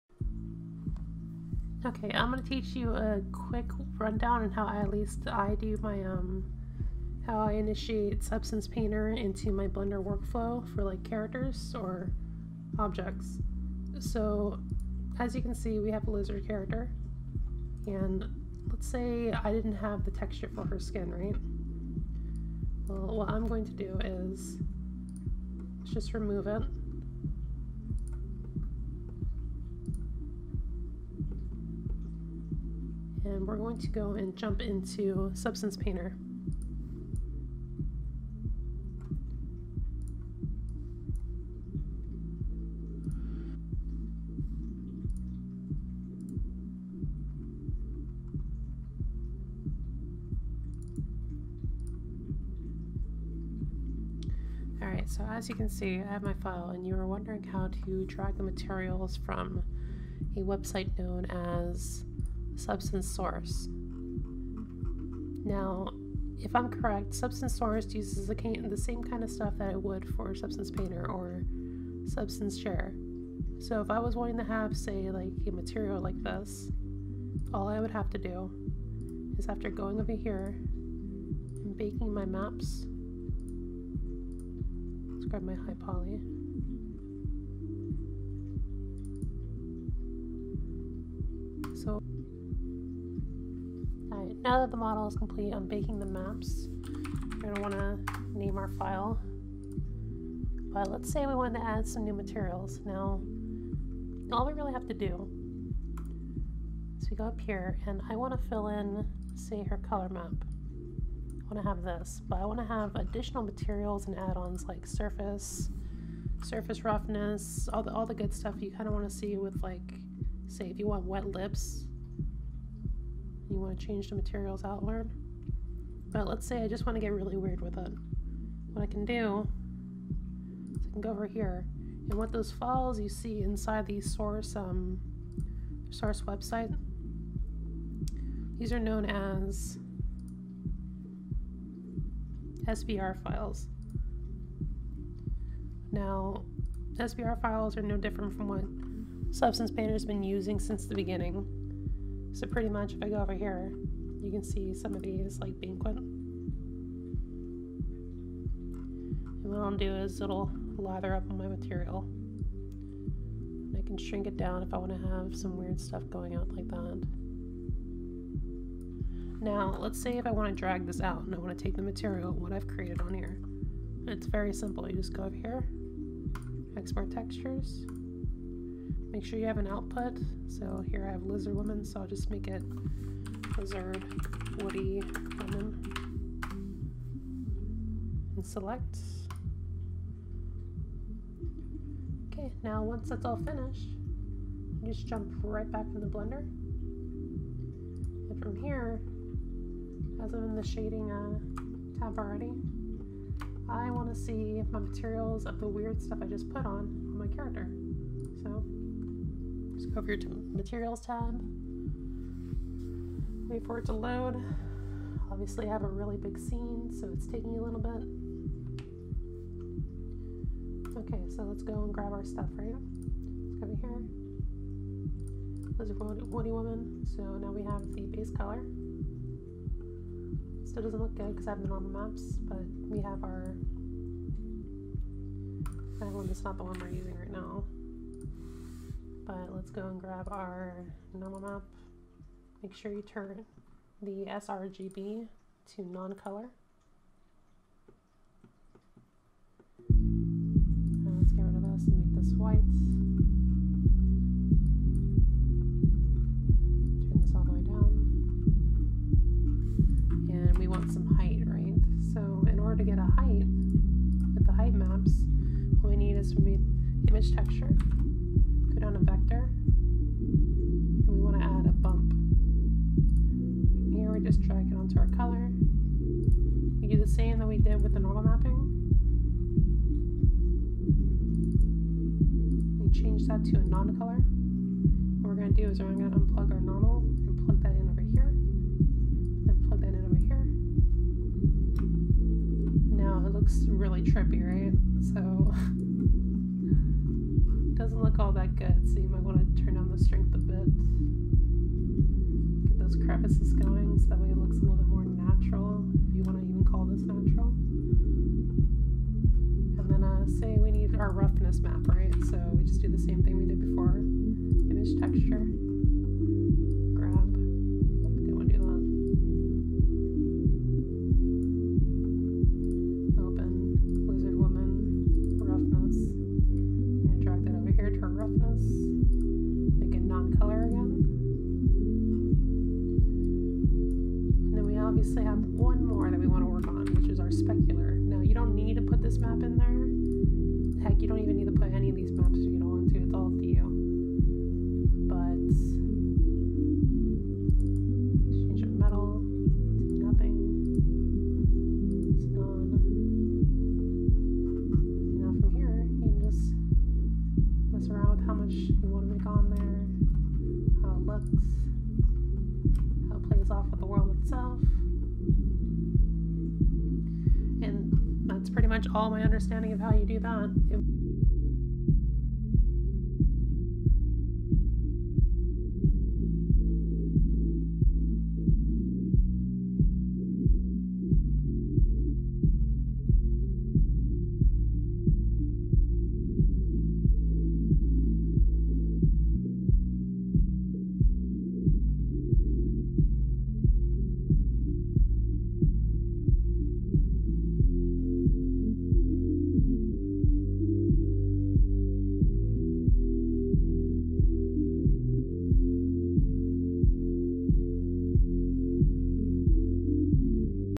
How to use substance painter with blender nodes - blender artists question
Now, this video might not be what was asked, but regardless its how i do what i do.

but if you are seeking a method to import SBSAR files from substance source to blender.

there was a addon to import them directly but nothing since 2.8 that i know of.

but here is the link to those videos anyways